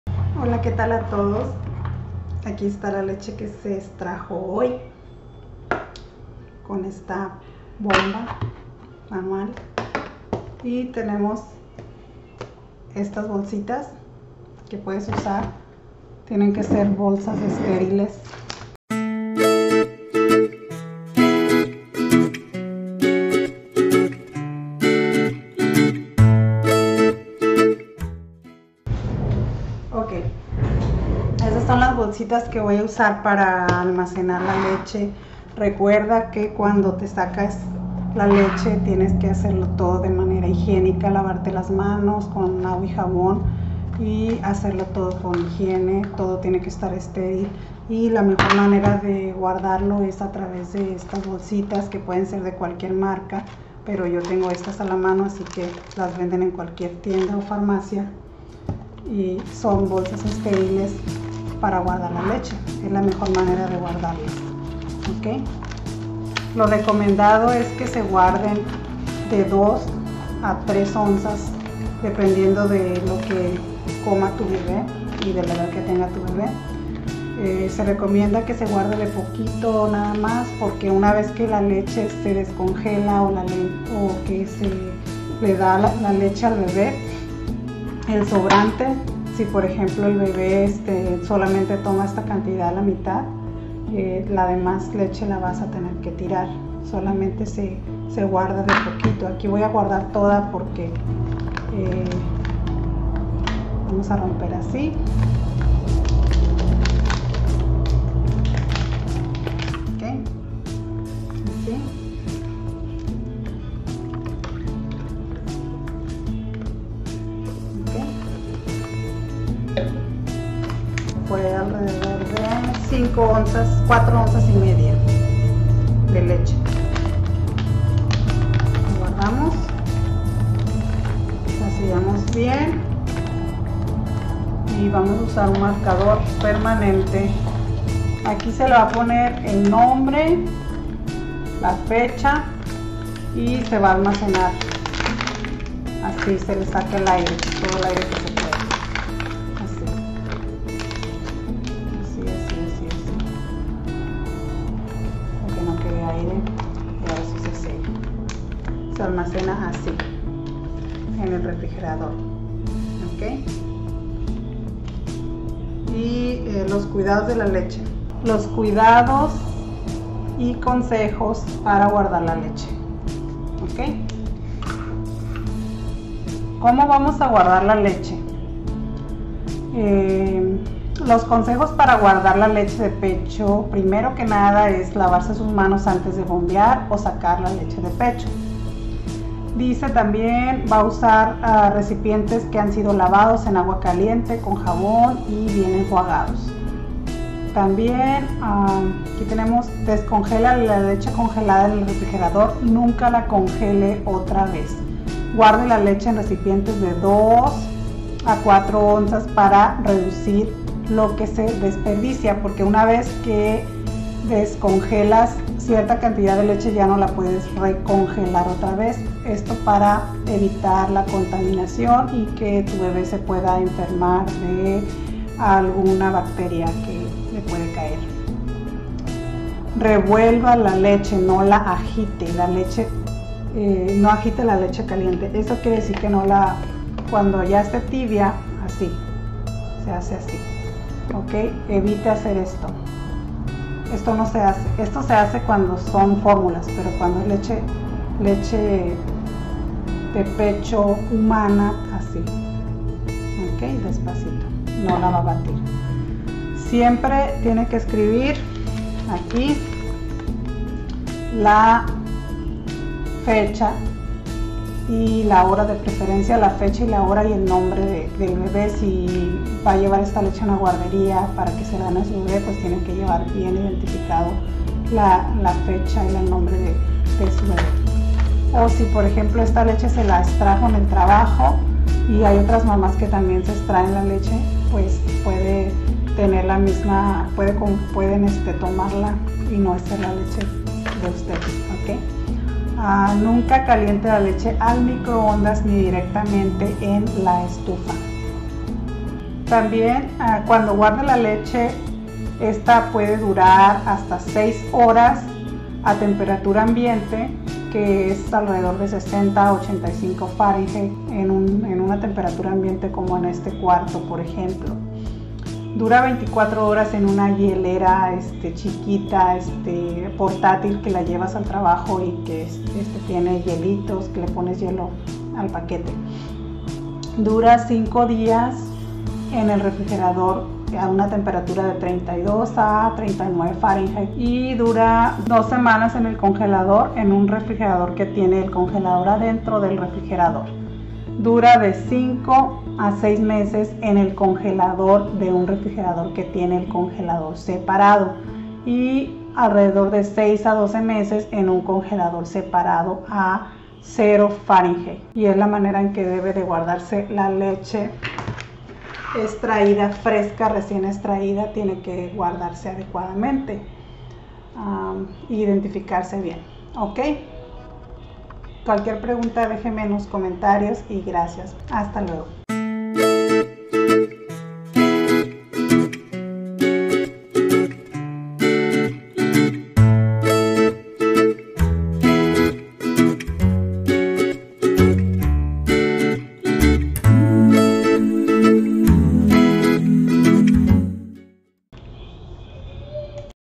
COMO GUARDAR LA LECHE DE PECHO RECIÉN EXTRAÍDA
Este video explica paso a paso como guardar la leche de pecho recién extraída, así como también las temperaturas adecuadas para almacenar la leche, y cuanto tiempo dura la leche de pecho.